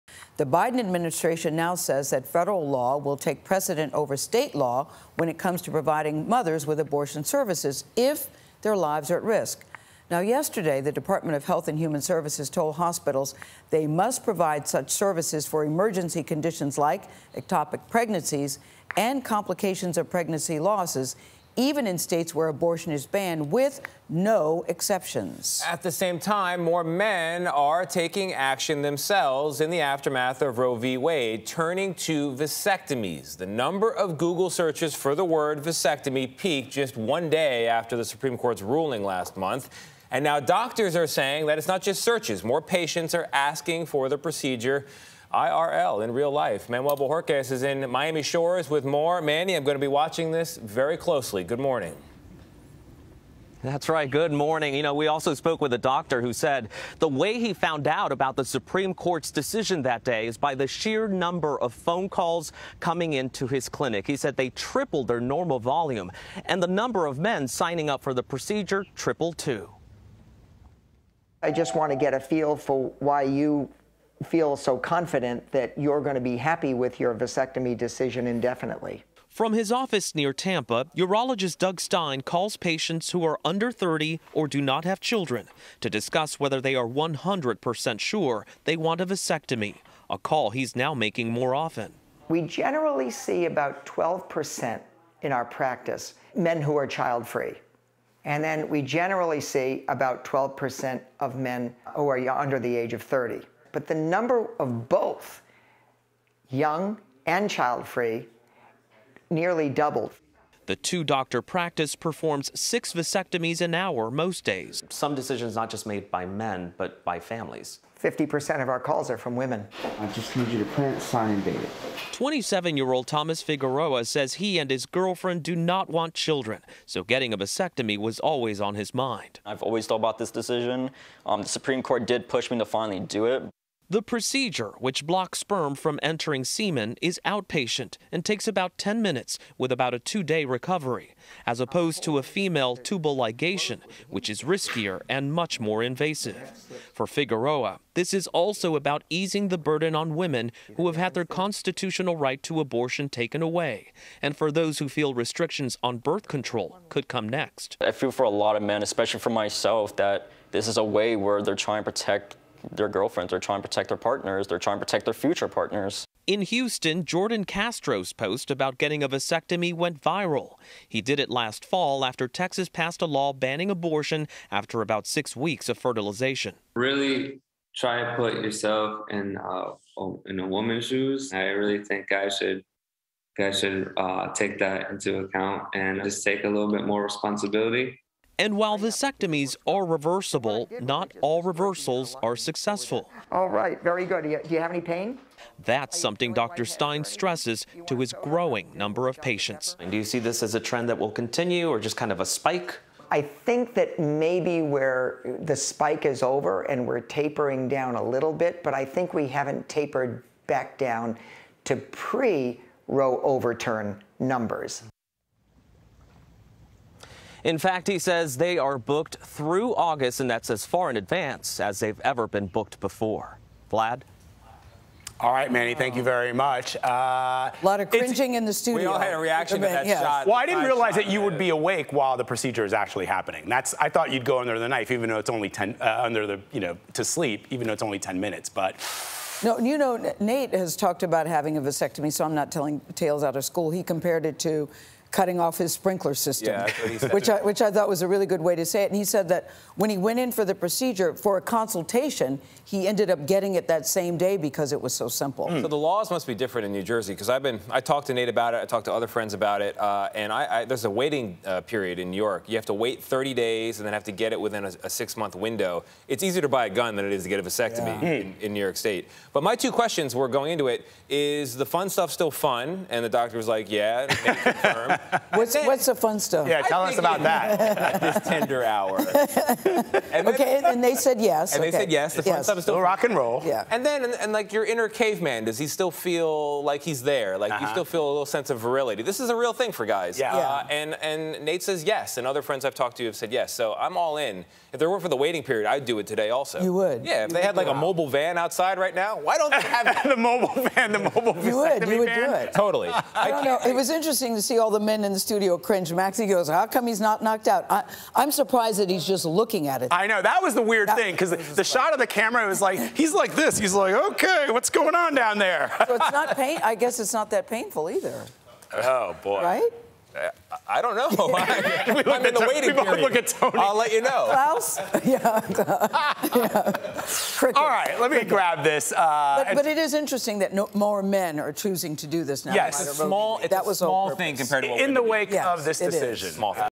More men seeking vasectomies after Roe v. Wade was overturned
After the Supreme Court overturned Roe v. Wade, signups for vasectomies spiked. Manuel Bojorquez was there as a young man went through the quick procedure.